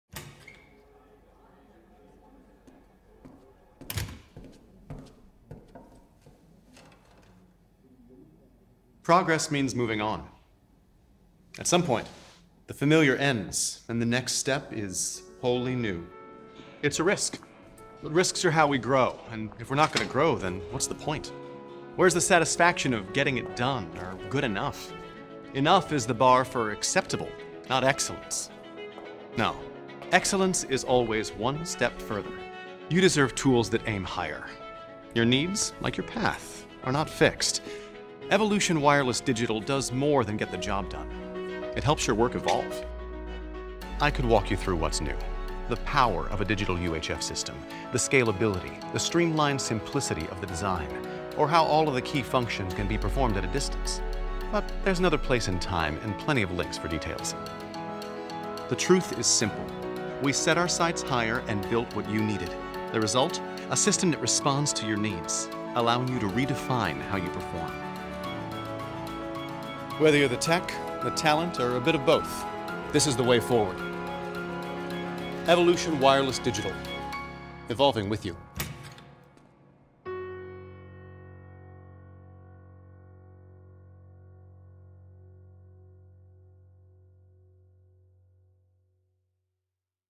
Evolution Wireless Digital – 새로운 디지털 무선 마이크 시스템 | Sennheiser
Evolving with you! Sennheiser Evolution Wireless Digital

모든 RF 환경에서 사용할 수 있는 디지털 UHF 시스템의 성능을 경험해보세요.
Evolution Wireless Digital은 간단한 설치 작업 및 안정적인 연결을 보장하며, 현재 시장에 나와있는 모든 무선 시스템 중, 가장 높은 다이나믹 대역을 제공합니다.
EW-D Smart Assist 앱을 통하여 모든 것을 간편하게 제어할 수 있습니다. Android 또는 iOS에서 앱을 설치하여 모든 시스템 설정에 대한 편리함을 경험해보세요.

최고의 사용 편의성, 사용자가 퍼포먼스에만 집중할 수 있도록 하는 새로운 디지털 무선 마이크 시스템, Sennheiser Evolution Wireless Digital 입니다.

아래 링크에서 더 자세한 정보를 확인하세요.

페이스북 & 인스타그램에서 젠하이저코리아를 팔로우해주세요: